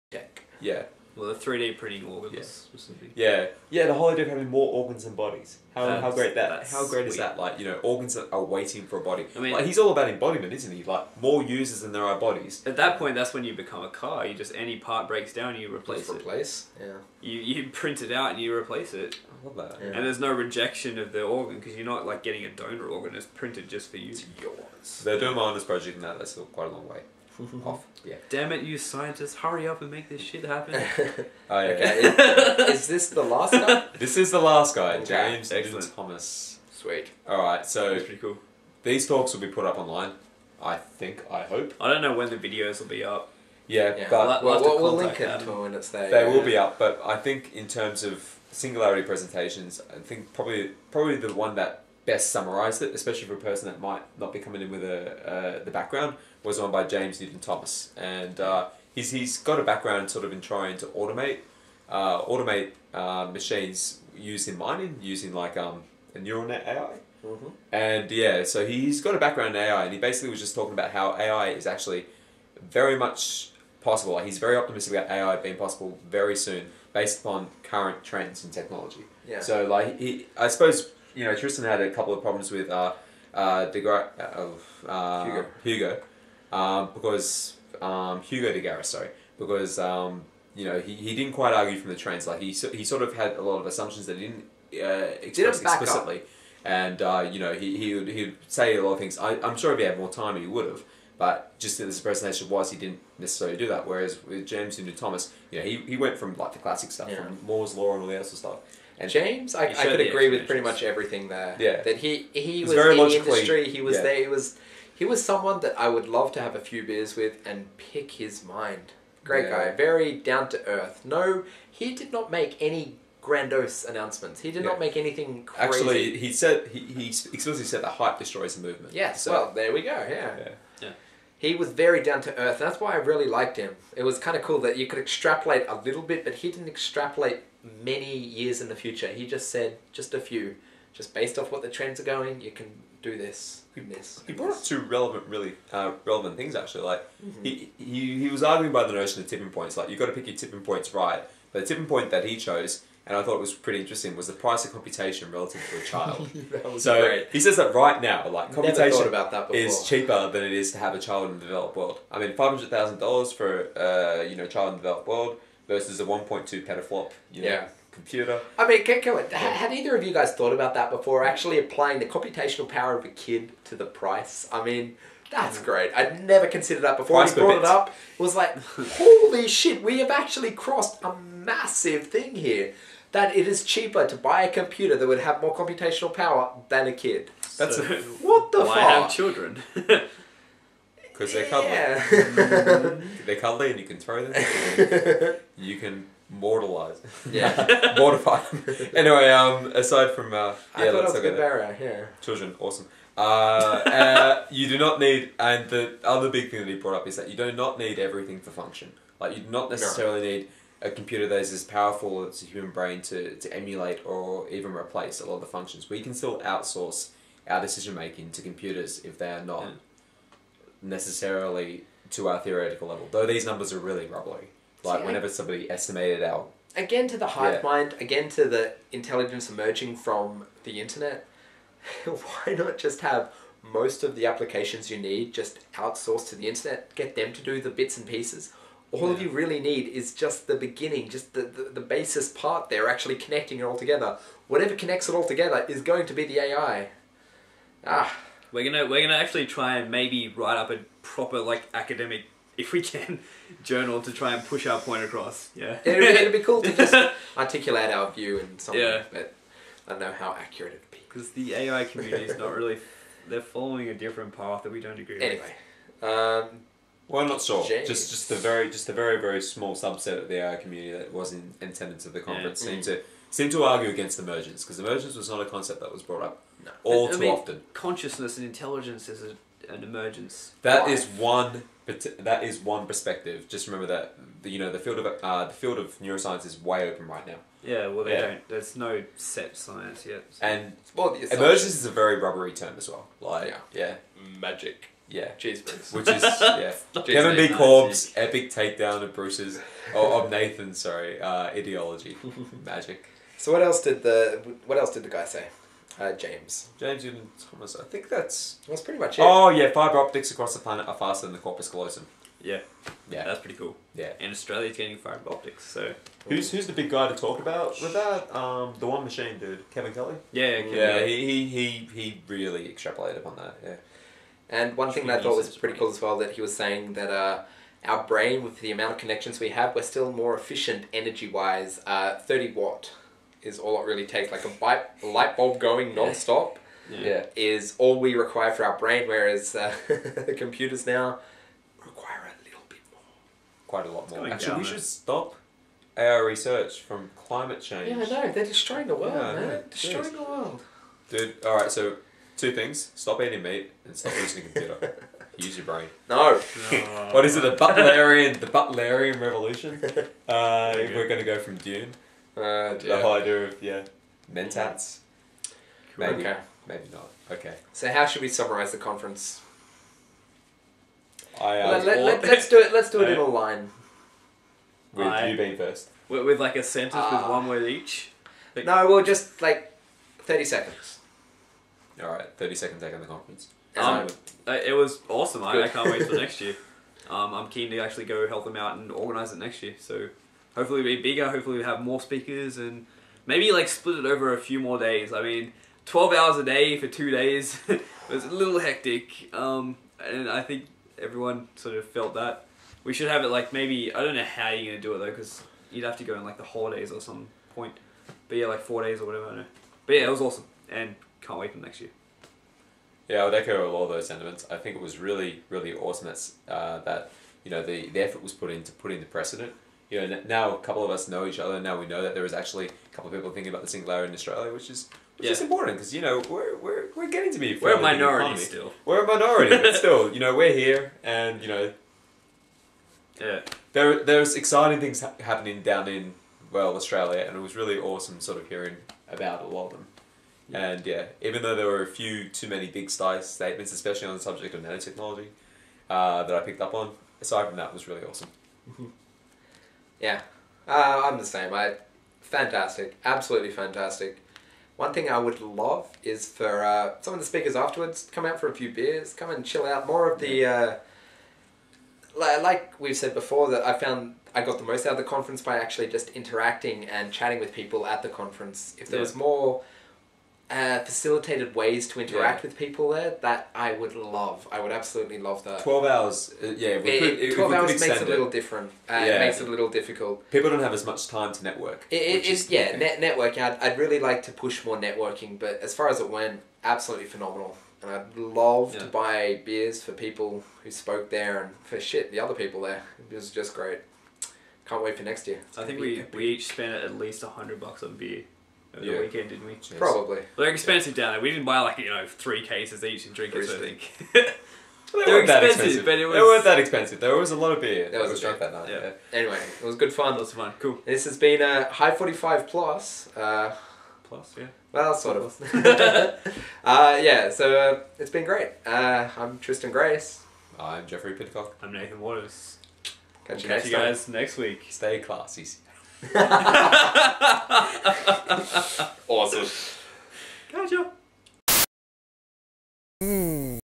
Hive45+ Podcast 4/4 - Singularity Summit Australia
HIVE45 PLUS+ episode featuring Nathan Waters, Geoffrey Pidcock and Tristan Grace for the week ending the 19/09/10 - wrapping-up our thoughts on the recent, inaugural Singularity Summit in Melbourne Australia.

The presentations we discuss:

* Robert Sparrow - "A Not-So-New Eugenics: The Harsh Logic Of Human Enhancement"

-- Liberal eugenics, Julian Savulescu, human enhancement via non-coercive market pressure, genetic modification, what defines "best", the criminal gene, picking the best genes for your children, the moral imperative is to upgrade and enhance, where do you define harm.

* Hugo de Garis - "AI -- An Artilect war or a Technological Utopia? "

-- The Artilect War (Artificial Intelligences) - the coming war between the Cosmists (those for building artilects) and the Terrans (those against), bigger than the war over capital ownership (communism vs capitalism), building intelligence orders of magnitude greater than our own - 10^24, becoming a cyborg, "Singularity" in the lexicon, robotics industry, quantum computers, Ed Witten, topological quantum computing, gigadeath - one billion deaths, Moore's Law is a historical generalization.

* Cameron Reilly - "The Singularity & Social Media"

-- A call to arms, teaching the public about the Singularity and exponential technological progress, grass-roots movement helps prevent subversion, exponential growth is important, we need a new term for "Singularitarian".

* Kevin B. Korb - "The Ethics of AI"

-- Ethical AI can be created, utilitarian AI, BDI agents (Belief-Desire-Intentions), "AI in 100s of years", AI from collective intelligence, philosophy of AI, building artificial intelligence with human ethics is "silly", talk of ethical AI merely to pander to the masses.

* Stelarc -- "The Cadaver, The Comatose and the Chimera"

-- Pushing transhumanism via art, machines inside body, growing organs on the outside - an ear on his arm, attaching his body to the Internet, robotic prosthetics, exoskeletons, bionics, actuated cybernetics, fractal flesh - what is a body?, body augmentation, third arm, 3D printing organs.

* James Newton-Thomas -- "Impact of the Singularity to the Citizen"

-- The Singularity impacts in simple terms, automation systems in mining, "hype destroys a movement", pick your tipping points - "computation is cheaper than a child in the developed world", don't need complete human brain replication for AI - can outsource in functional terms, outsourcing to the Internet, fiber optics across the planet is faster than the Corpus callosum, brain is very energy efficient (approx 30W constant).